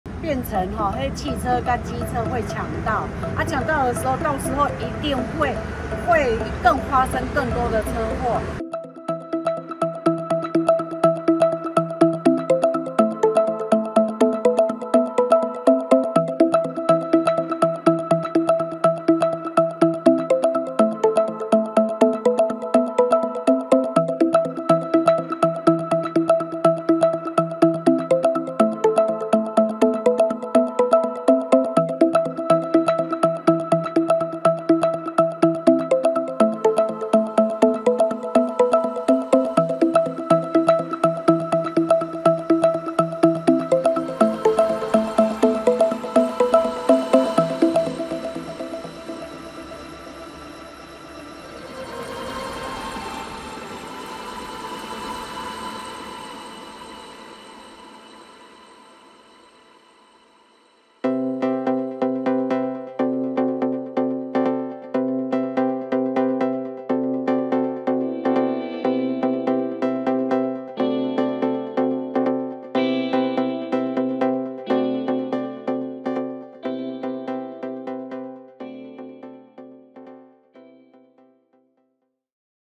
彰化六岔路口新設「槽化線」議員怒轟易車禍？網全讚護行人！｜👍小編推新聞 20231218
彰縣府交通處表示，設計都有經過審查。〈求收編〉